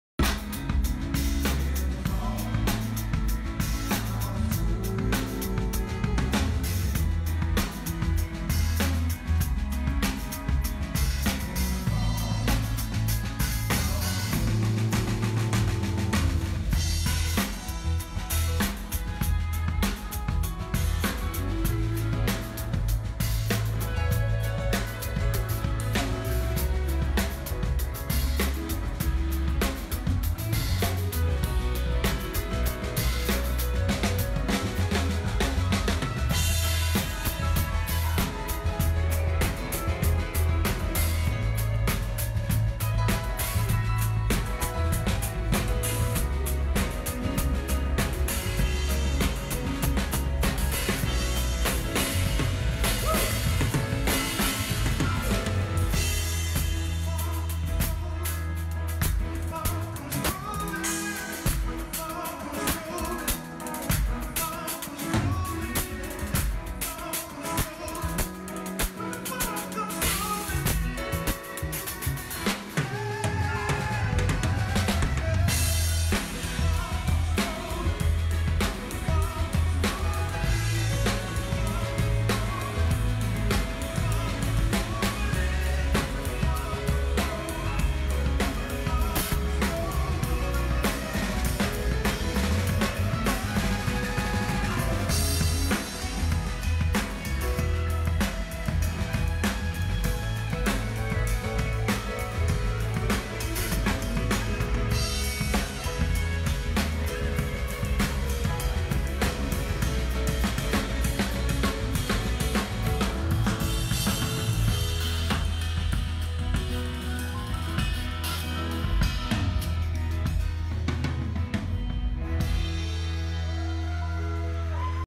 CEE JOHN DRUM. John Calarco - The Cooperage - 2018
John Calarco with Keith Pulvermacher. The Cooperage 2018. Video by Robert Flessas.